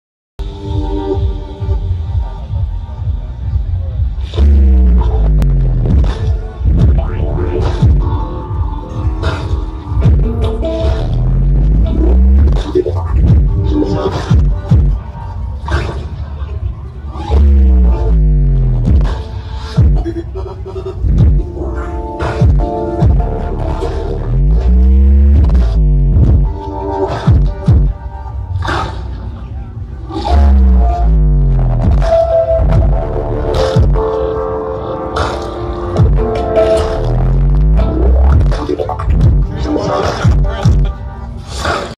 Lighting and visuals by Fractal Planet crew.
The Fractal Planet visuals and lighting crew was unstoppable this year. So much of our shit got eaten by the playa, but we would not, could not stop.
Fractal Planet 2013; 2:00 Esplanade